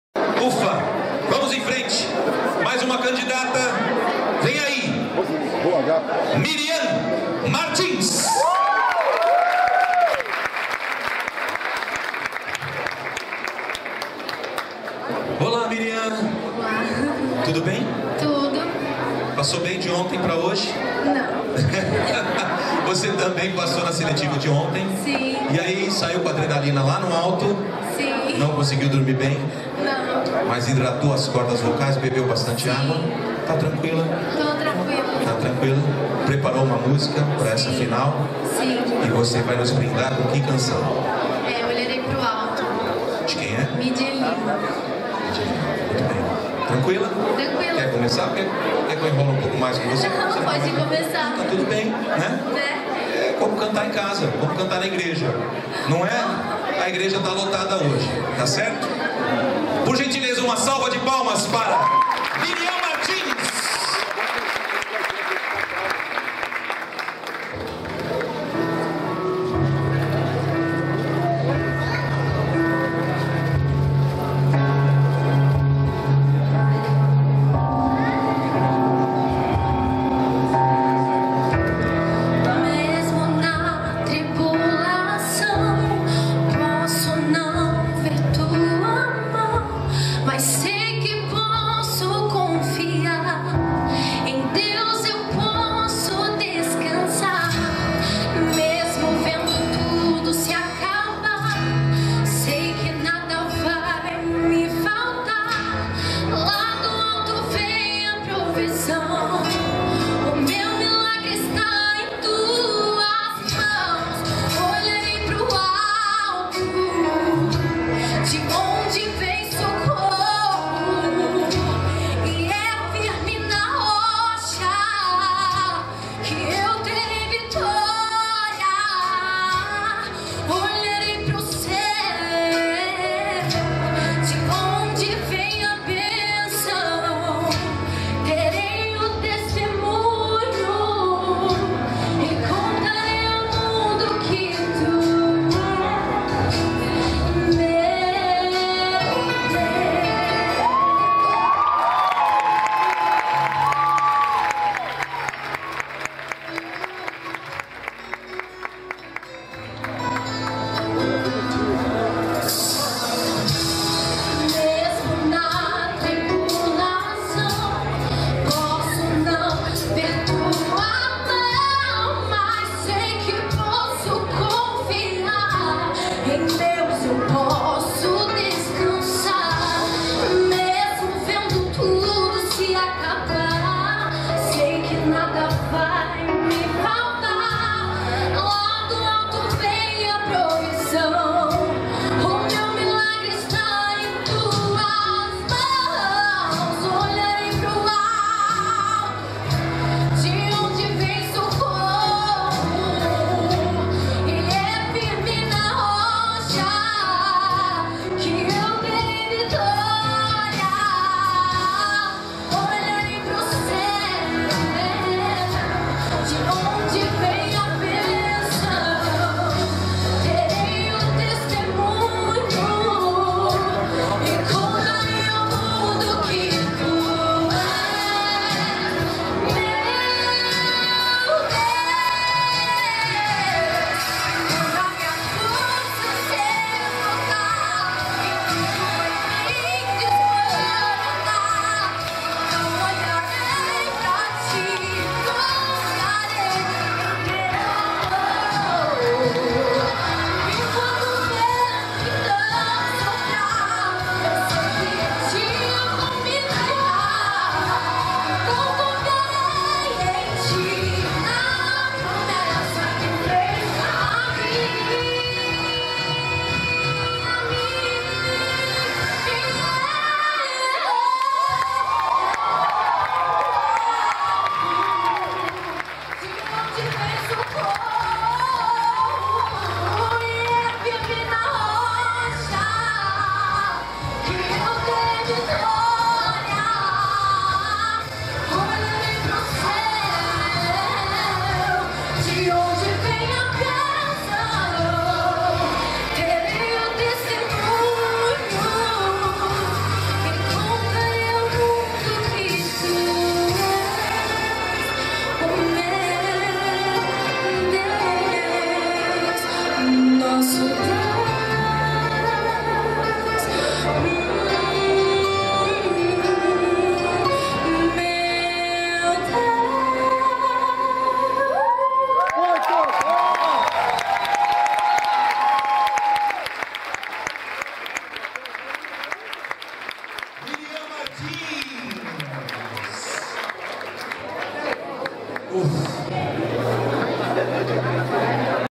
CAMPEONATO MUNDIAL DE KARAOKÊ BRASIL 2019
Mirian Martins campeã da seletiva do Campeonato Mundial de Karaokê em Caraguatatuba - SP